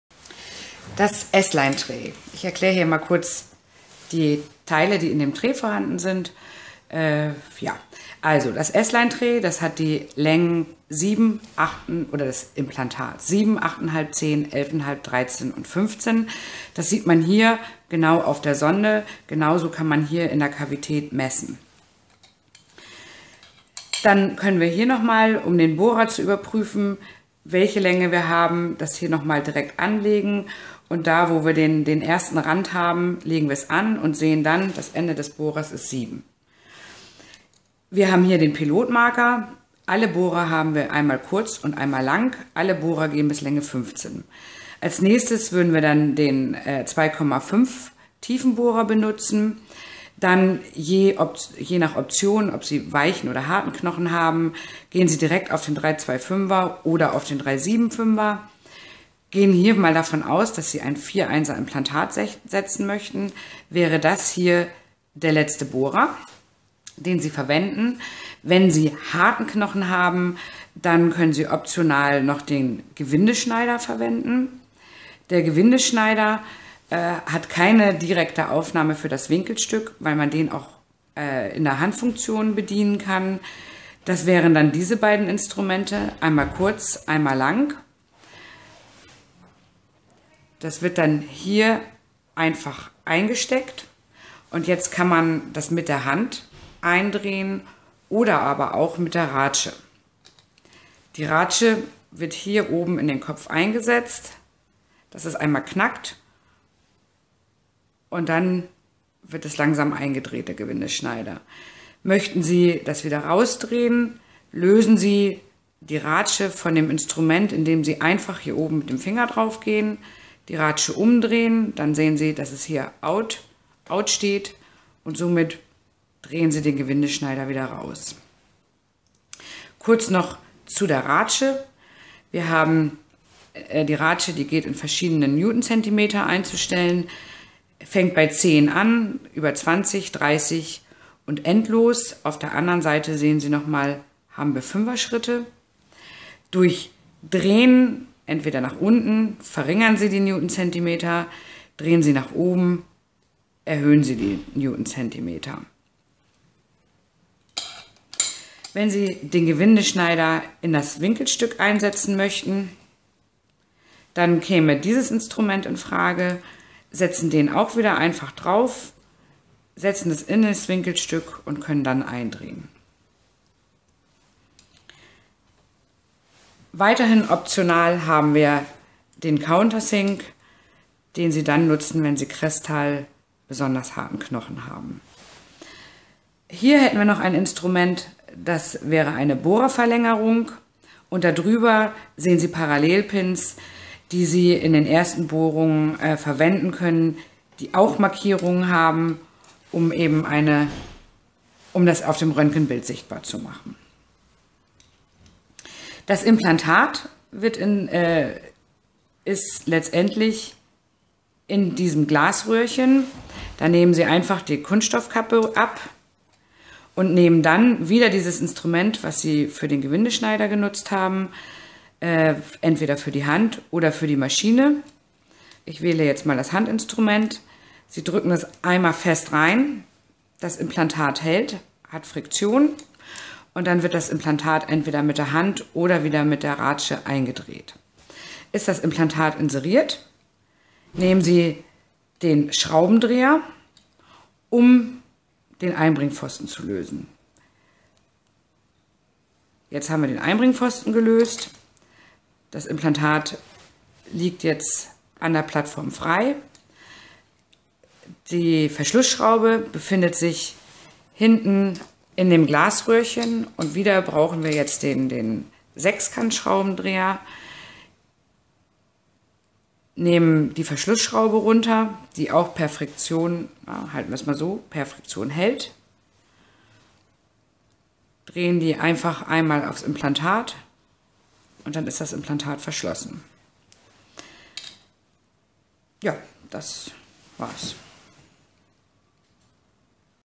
Implantat Chirugie Instrumente Bego S-Line
Handhabung Instrumente zum Einbringen der Bego S-Line Implante